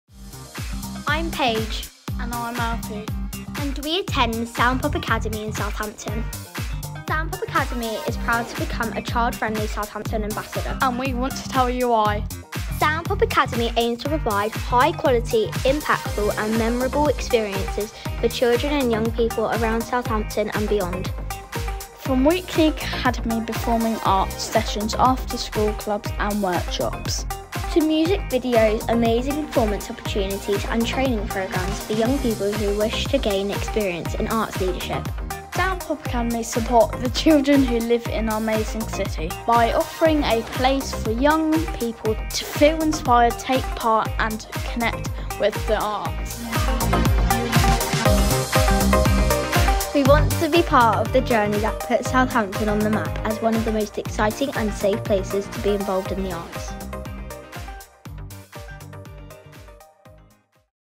Sound Pop Academy - Child Friendly Southampton
Here at Sound Pop Academy, we're proud to be a Child Friendly Southampton Ambassador!